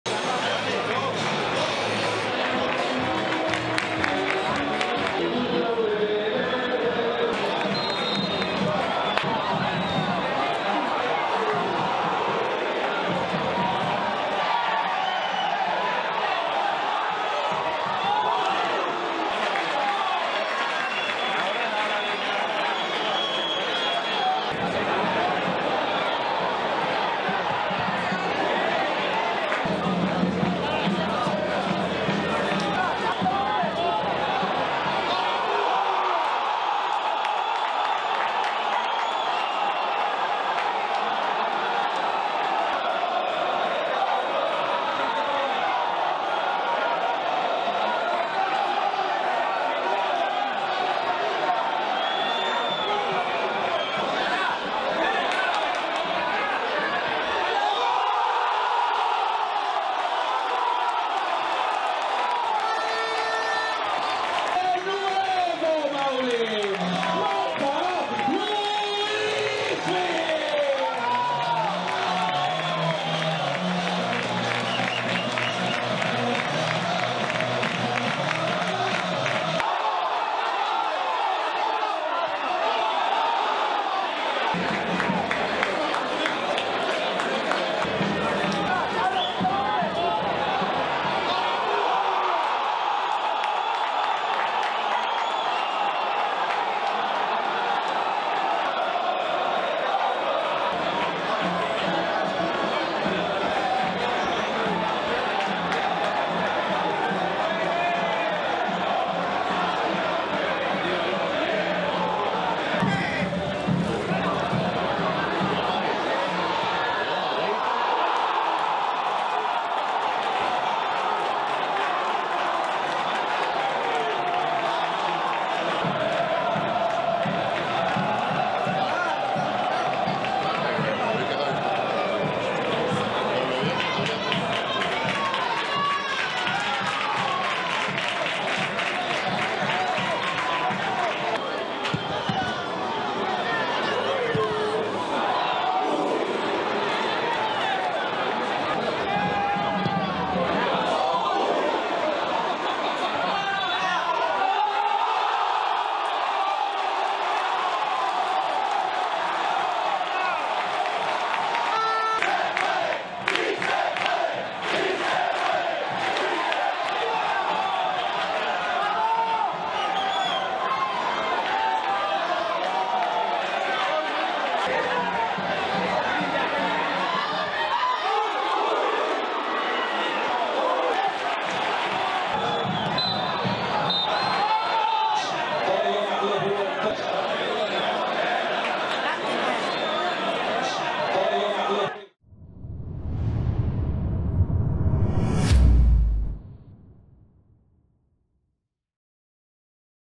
Resumen PrimeraFederación | Antequera CF 2-3 Córdoba CF | Jornada 26, Grupo 2
⚽️⚽️ Antonio Casas
⚽️ Luismi Redondo
⚽️ Diarra
⚽️ Fomeyem

📍 El Maulí